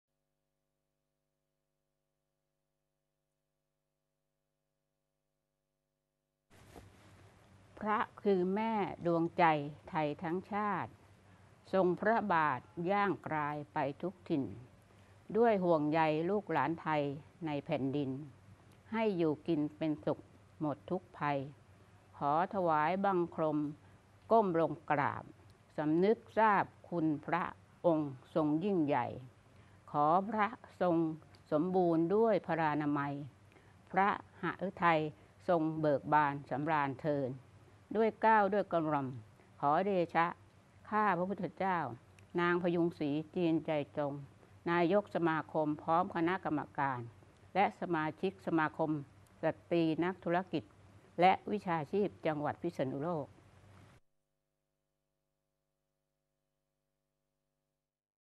สมาคมสตรีนักธุรกิจและวิชาชีพพิษณุโลก
ถวายพระพรสมเด็จพระนางเจ้าสิริกิติ์ พระบรมราชินินาถ พระบรมราชชนนี พันปีหลวง เนื่องในวันเฉลิมพระชนพรรษา 87 พรรษา
บันทึกเทปเมื่อ   23 กรกฎาคม 2562